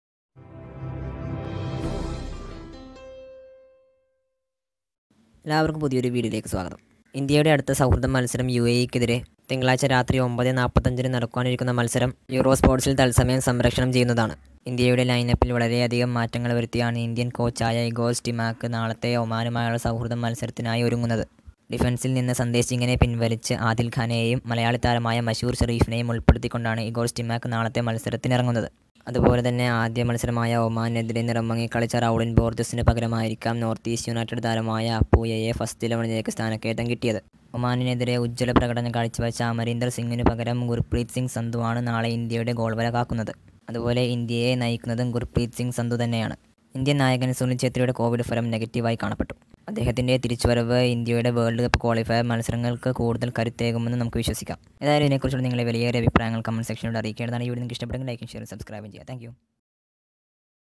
Kerala Blasters | Jhingan കളികുന്നില | മലയാളി താരം കളിക്കും | ചെട്രിയുടെ covid
1- kerala blasters players
2- kerala blasters strikers